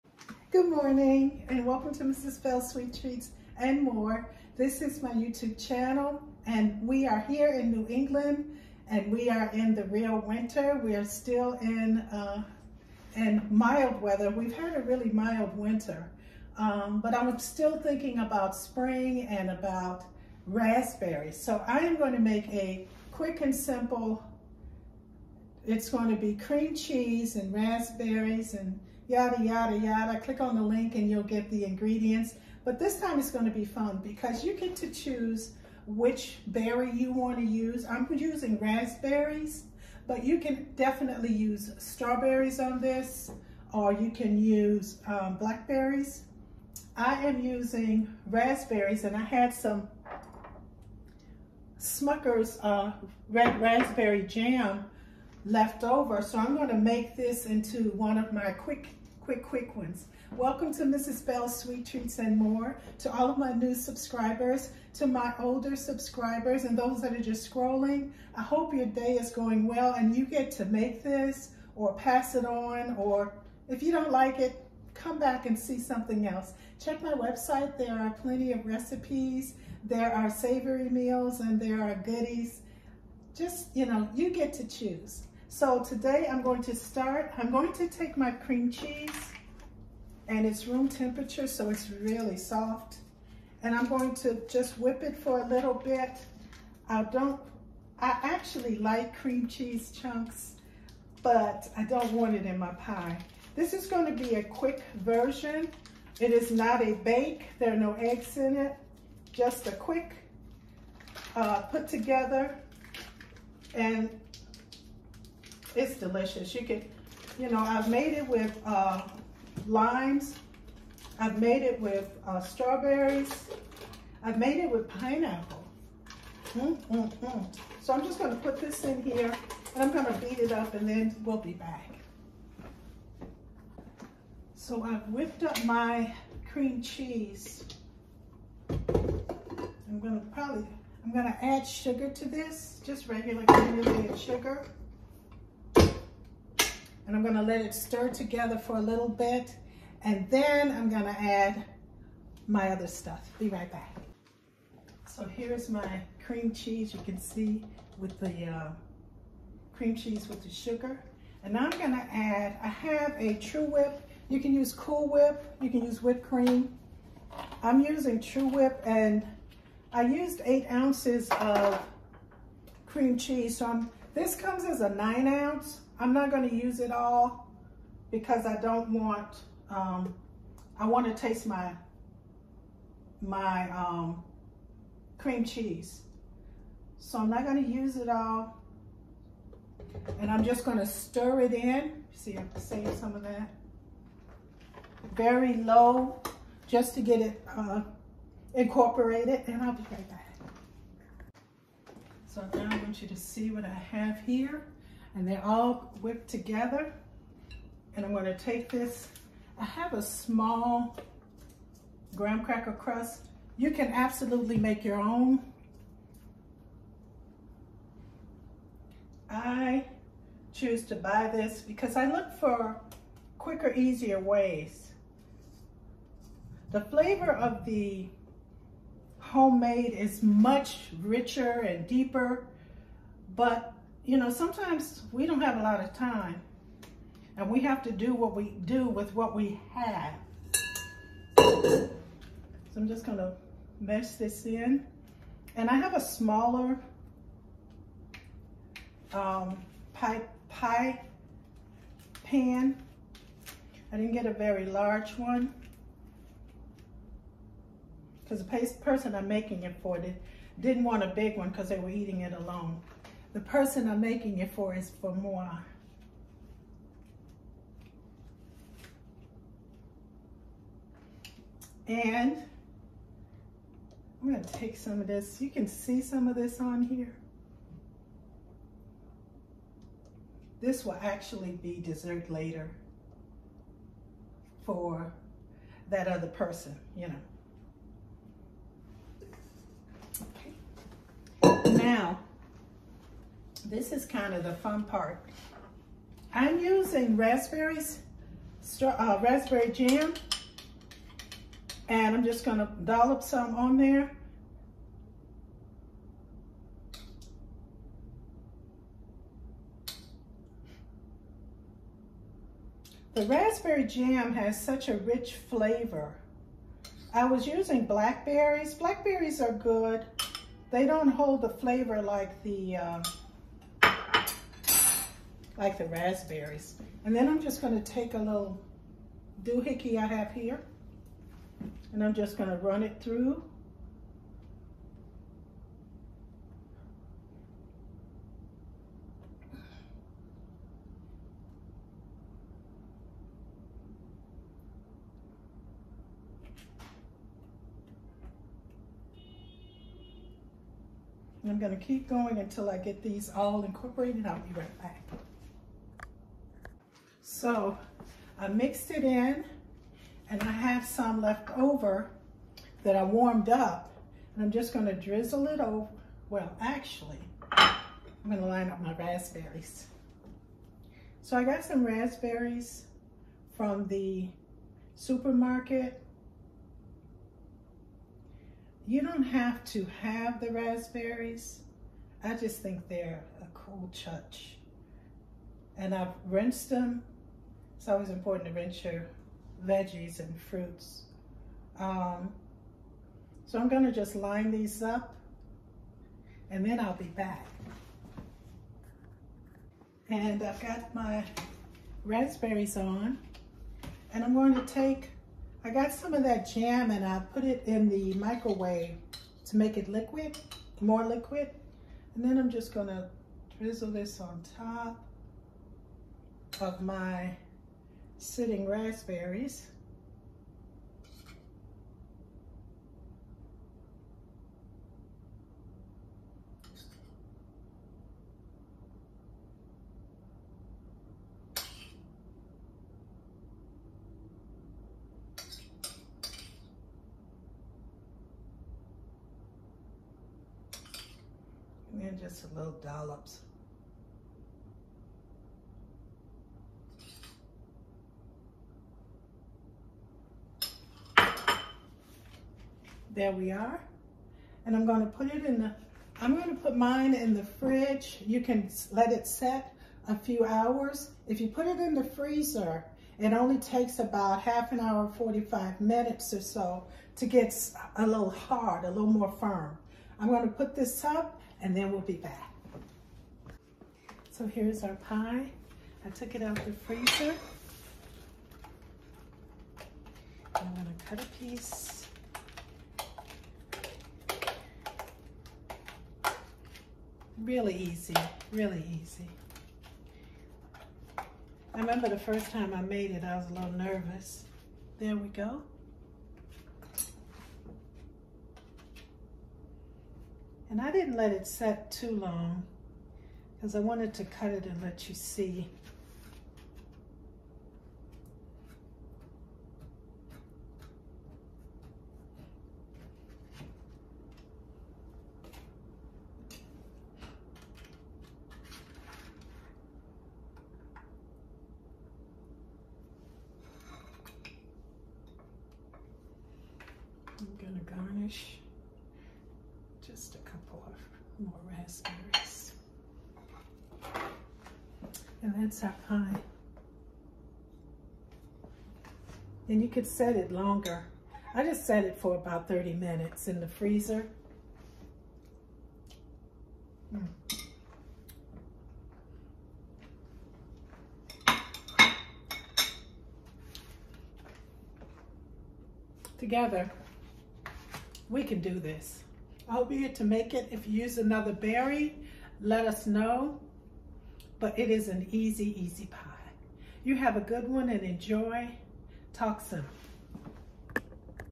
Monday Moments: Mrs. Bell's Easy Raspberry Cream Cheese Pie
Mrs. Bell's Easy Raspberry Cream Cheese Pie
Stand alone or hand mixer (optional)
1 ready-made graham cracker crust

Ingredients:

One-8 ounce cream cheese (softened)
One-8 ounce container of cool whip, TruWhip (thawed)
1/3 cup granulated sugar
½ cup *raspberry jam (divided)
*Raspberries for garnish (optional)

Instructions:

On medium speed whip together cream cheese and sugar until well combined

Stir in half the raspberry jam then fold in Cool Whip or TruWhip

Scoop the mixture and place into the graham cracker crust, replace the lid and chill about 2 hours in fridge

Garnish with fresh berries (optional)

Chill in fridge for a couple of hours or in freezer for an hour

Serve chilled and enjoy!

*Fat free and low fat cream cheese works well
*Raspberry, strawberry, or blackberry jam